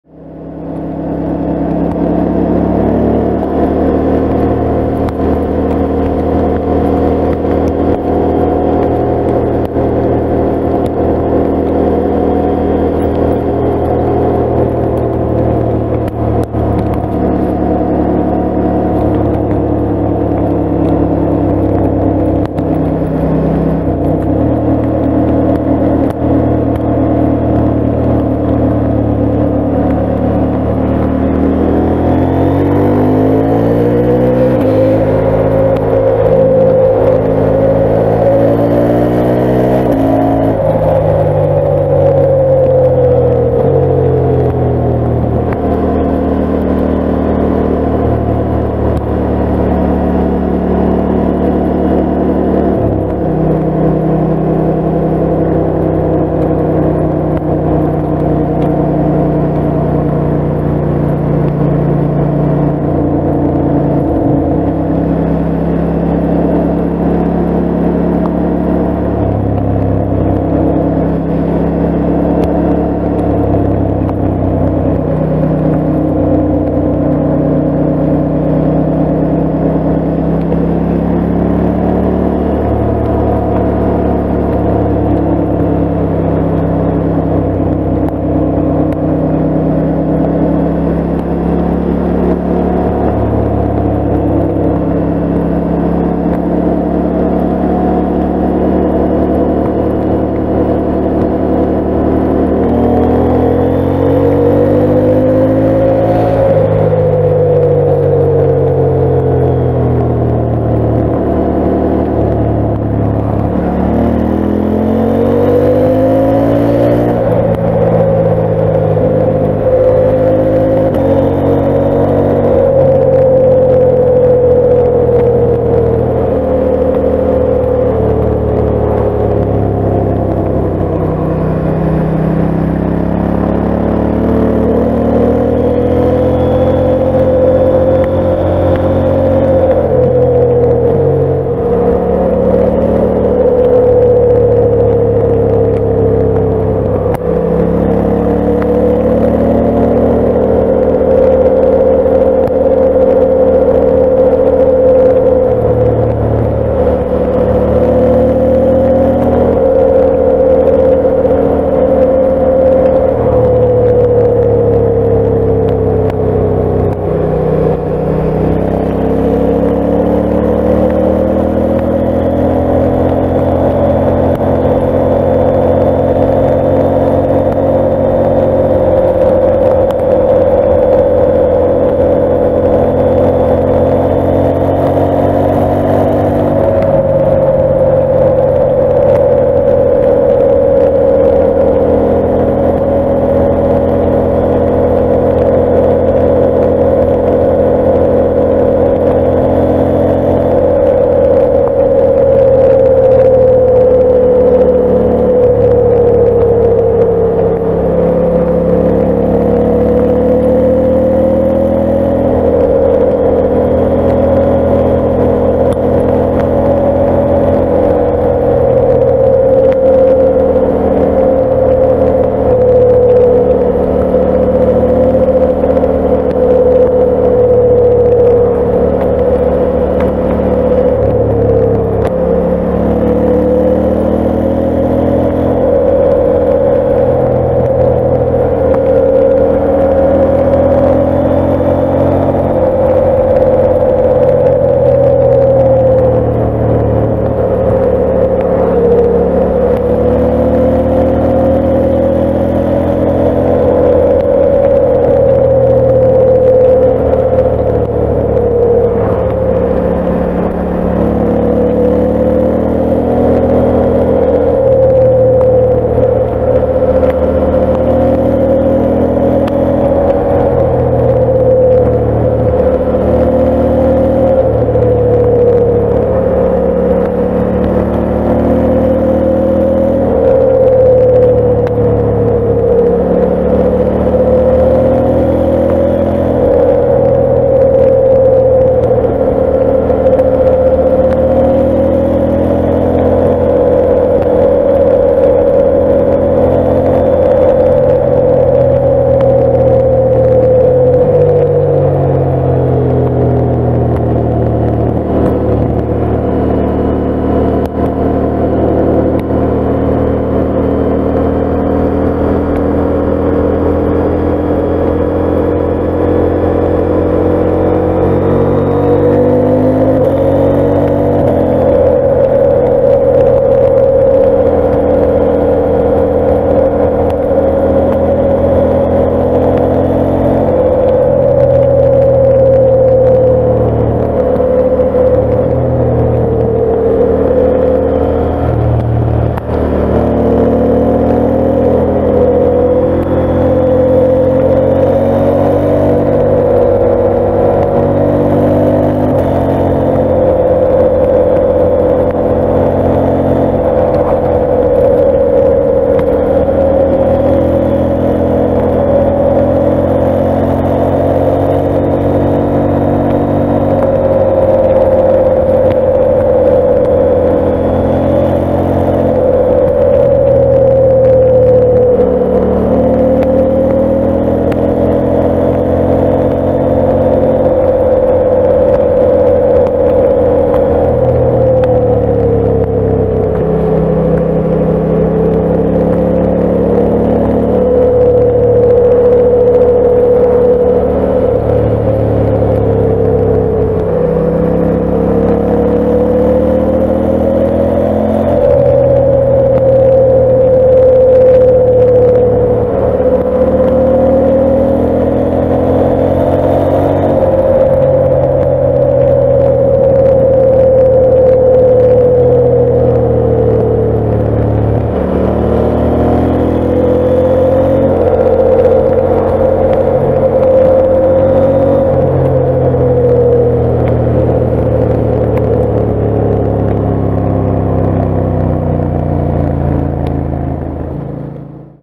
Karoo 4 review part 5, Faster pace, evaluation
Part 5 of my long term review of KAROO 4 tires on my KTM 890 Adventure. Today I am going to share my thoughts about this tire so far while riding the tire at quite a pace.

So let´s dive into Metzeler KAROO 4 review part 5!